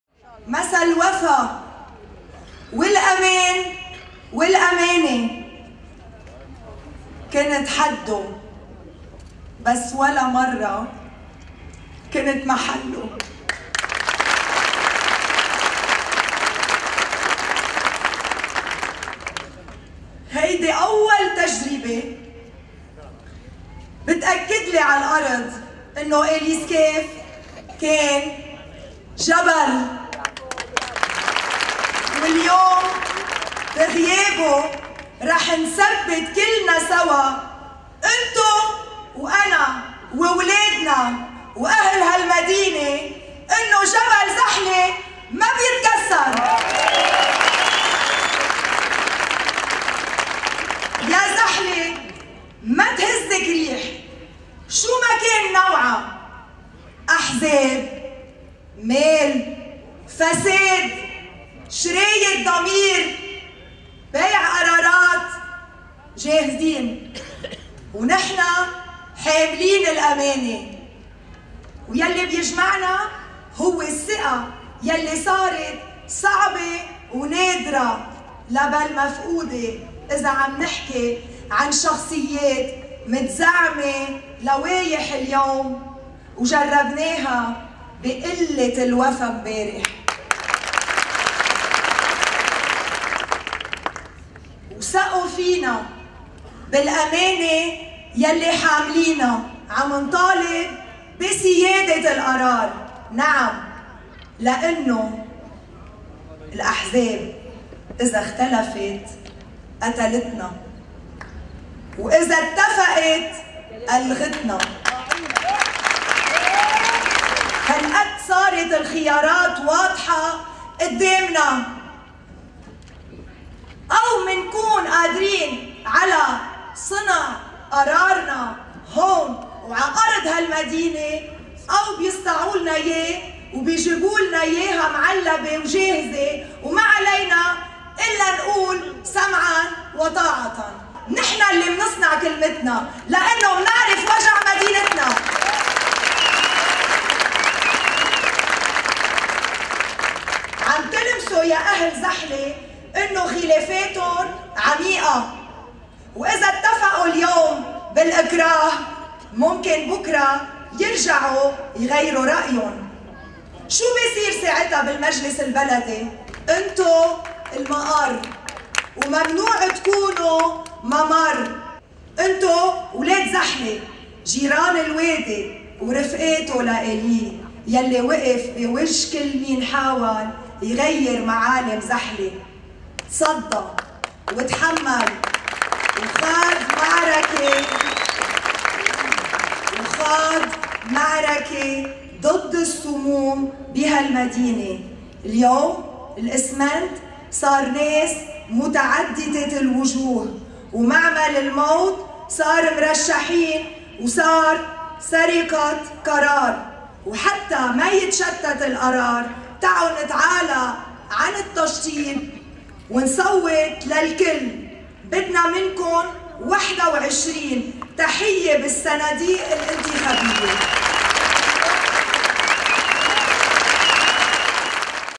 كلمة رئيسة الكتلة الشعبية ميريام سكاف خلال إعلان لائحة "زحلة الأمانة"
مسا الوفا
والامان .. والامانة
كنت حدو .. بس ولا مرة كنت محلو
هيدي اول تجربة بتأكدلي ع الارض انو أيلي سكاف كان جبل
واليوم بغيابو .. رح نثبت كلنا سوا .. انتو وانا وولادنا وأهل هالمدينة أنو جبل زحلة ما بيتكسر
ويا زحلة ما تهزك ريح
شو ما كان نوعها
أحزاب .. مال .. فساد .. وشراء ضمير .. بيع قرارات ..
جاهزين ..ونحنا حاملين الامانة
لانكن اهل الوفا.. تركلكن امانة
لانو الصدق صديقكن.. تركلكن امانة
لانو الشهامة ساكنة زحلة ..تركلكن امانة
لانو هوي بيناتنا وما في شي بيبعدو .. مدينتو صارت امانة بضميرنا
وأمانتك يا ايلي انو نخلي قرارنا بقلب زحلة
ونوصّل صوتا بحناجر ولادا
من هون انطلق شعارنا .. شعار اللائحة الي بيحمل عنوان : زحلة الامانة .
هيدي مش كلمة بتزيّن طريق
مش شعار لمرحلة بلدية وبس
هيدا خط ونهج ووصية وعهد .. ..وكلنا بيعرف قديه صعبة أنو نخون الامانة او ما نكون قدها
ونتشتت وتصير أصواتنا ورقة بتنشرى وبتنباع بسوق المحشورين ع صوت .
يا إيلي الي فوق .. ناسك هون .. قدّها وأكتر ..
ناسك من طينة شعب صلب .. بيمشي عطريق وبيكفيها .. ما بيترك .. ما بيزيح ..
وبيصير أقوى كل ما أشتد التحدي ..
والي بيجمعنا .. هوي الثقة الي صارت صعبة ونادرة
لا بل مفقودة إذا عم نحكي عن شخصيات متزعمة لوائح اليوم ..وجربناها بقلة الوفا مبارح ..
وثقوا فينا .. بالامانة الي حاملينا ..
وكرمال هيك كنا أول فريق إنتخابي بزحلة بيتمكن من تأليف لائحة بلدية بلا خلافات ..من دون أزمات .. بلا عرقلة
لأنو كان قرارنا بإيدينا .. ومش محتاجين لنستوردوا من خارج المدينة .
كلنا فخر باعلان لائحة : زحلة الامانة الي بتجمع عائلات هالمدينة ومكوّناتها ..
وبتضم الخبرة مع الشباب . النضج مع التطور .. . والاهم أنها على صورة الناس " الاوادم " الي بيكفوا طريق " الادمي" الاول .
ويا ريت فينا بهاللائحة نمثل كل عائلات المدينة .. وطبعاً بدنا نبدي الأسف أنو في شخصيات ما قدرت تكون معنا لأنو بالاخر محكومين بعدد محدد وبتمثيل لجميع الطوائف . ويا ريت كلن بيكونوا معنا .. بس الاكيد انو كلن بقلبنا.
قدرنا توافقنا ع أسمائنا بلا ما نستشير أصحاب القرار من برا ..
قدرنا نرسم خط إنمائي بدو يشتغل لصالح المدينة ..وبيتعامل معها كأنها جمهورية مستقلة عن الاحزاب ومش عن الدولة ..
عم بنطالب بسيادة القرار .. نعم
لأنو الاحزاب إذا إختلفت.. قتلتنا
وإذا إتفقت الغتنا ..
ومع هيك ومن حرصنا على انو نكون سوا .. حاولنا جهدنا نروح عالتوافق ونلّم شمل المدينة
لكن المعادلة ما بتمشي على قاعدة الغاء أو تحجيم أحد أهم مكونات نسيج المجتمع الزحلي وهوي: الكتلة الشعبية يلي عمرها بيفوق اعرق الاحزاب . . الكتلة الموجودة بضمير ووجدان زحلة .
- مين من الاحزاب رح يعرف شو بكن ؟
-مين عاش مع وجع الناس يوم بيوم وحتى من دون ما يكون بالسلطة ؟
-مين رح يكون ضمير حي ليقول كلمة حق ويقود زحلة إنمائياً ع فرد قلب واحد ؟
- مين غير الكتلة الشعبية بيهمها همومكن ؟
- مين غيرها ..هالكتلة الي كلها إيمان بالله وفيكن .. وما بتحتمي بالرب بهدف السطو ع الارض ؟
ع كل هالعناوين بنلتقي ..
وإذا ما توافقنا مع غيرنا من الاطراف مش يعني صرنا أعداء .. هيدي مرحلة إنتخابية مسموح فيها إستعمال كل قوتنا ..بس مش بالطرق السودا ..